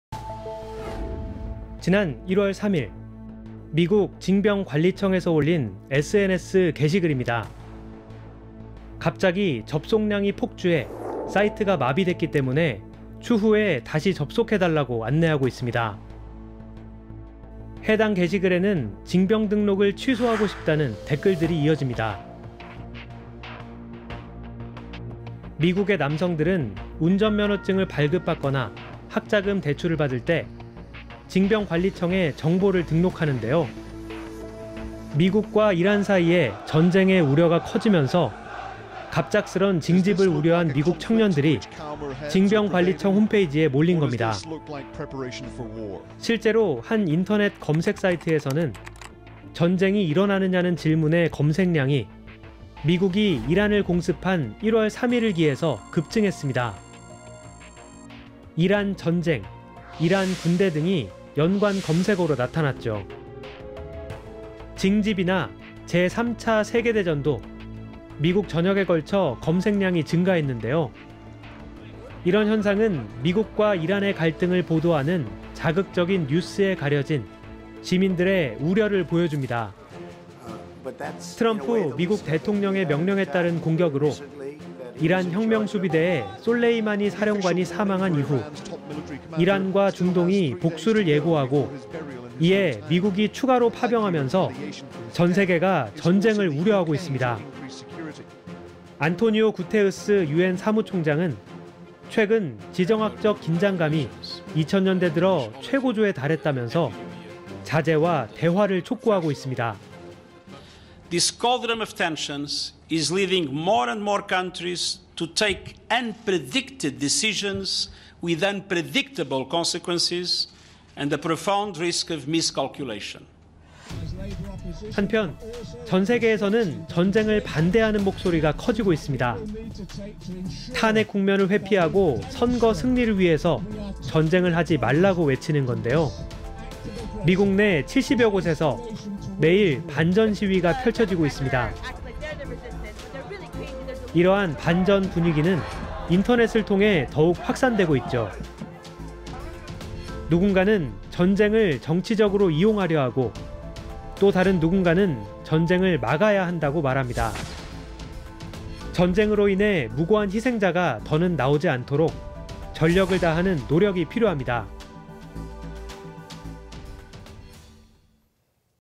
2020.01.09 [뉴스G] 인터넷 검색어가 말하는 것
지난 1월 3일, 미국의 ‘징병 관리청’에서 올린 SNS 게시글입니다.
‘갑자기 접속량이 폭주해 사이트가 마비됐기 때문에 추후에 다시 접속해 달라’고 안내하고 있습니다.
해당 게시글에는 ‘징병 등록을 취소하고 싶다’는 댓글들이 이어집니다.
미국 남성들은 운전면허증을 발급받거나 학자금을 대출 받을 때 징병 관리청에 정보를 등록하는데요,
미국과 이란 사이에 전쟁 우려가 커지면서 갑작스런 징집을 우려한 청년들이 징병 관리청 홈페이지로 몰린 것입니다.
실제로 한 인터넷 검색 사이트에서는 “전쟁이 일어나느냐?”는 질문의 검색량이 미군이 이란을 공습한 1월 3일을 기해서 급증했습니다.
‘이란 전쟁’, ‘이란 군대’ 등이 연관 검색어로 나타났습니다.
‘징집’이나 ‘제3차 세계 대전’도 미국 전역에 걸쳐서 검색량이 증가했는데요,
이런 현상은 미국과 이란의 갈등을 둘러싸고 보도되는 자극적인 뉴스에 가려진 시민들의 우려를 보여줍니다.
트럼프 미국 대통령의 명령에 따른 공격으로 이란혁명수비대의 솔레이마니 사령관이 사망한 이후에 이란과 중동이 복수를 예고하고, 이에 미국이 추가로 미군을 파병하면서 전 세계가 전쟁을 우려하고 있죠.
안토니오 구테흐스 유엔 사무총장은 ‘최근 지정학적 긴장감이 2000년대 들어서 최고조에 달했다’면서 자제와 대화를 촉구하고 있습니다.

안토니오 구테흐스 유엔 사무총장
“이런 상황에서 더 많은 국가가 예측 불가능한 결정을 내릴 경우 예측 불가능한 결과와 중대한 오판이 위험으로 이어질 수 있습니다.”

한편, 전 세계에서는 전쟁을 반대하는 목소리가 커지고 있습니다.
탄핵 국면을 회피하고 선거 승리를 위해서 전쟁을 하지 말라고 외칩니다.
미국 내 70여 곳에서 매일 반전 시위가 펼쳐지고 있습니다.
이런 반전 분위기는 인터넷을 통해 더욱 확산되고 있습니다.
누군가는 ‘전쟁’을 정치적으로 이용하려 하고 또 다른 누군가는 ‘전쟁’을 막아야 한다고 말합니다.
‘전쟁’으로 인해 무고한 희생자가 더 이상 나오지 않도록 전력을 다하는 노력이 필요합니다.